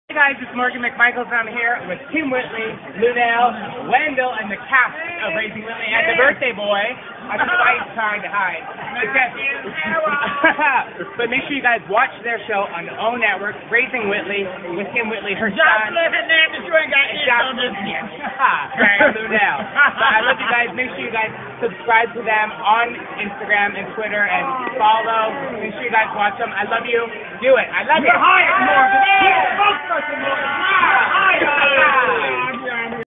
Chilling with the cast of RAISING WHITLEY !!!
I got the great opportunity to make a cameo on the upcoming season of Raising Whitley on the OWN network and we are chilling out having a drink after the filming was done ! I got to meet  Kim Whitley , Lunell and always fun seeing my friend Wendell James !!!